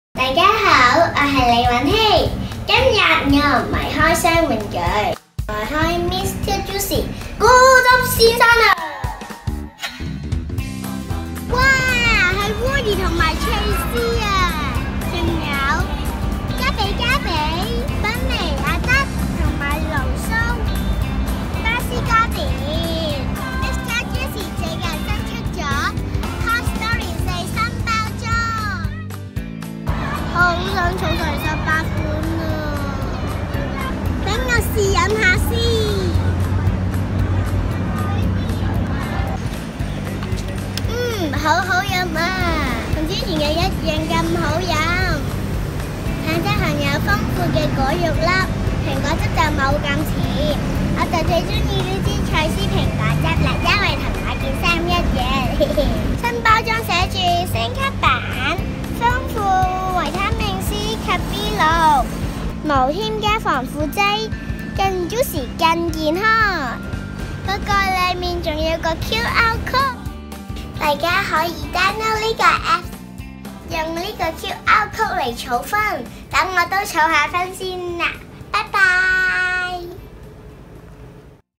【小網紅大賽2019】初小組李尹曦入圍作品
「小網紅大賽」現正進行得如火如荼，而各位小朋友都學有所成，各自拍完剪好條超晒班嘅開箱片喇！依家快啲嚟睇下咁多位參賽者嘅畢業短片，一齊投票揀出「我最Like小網紅」啦！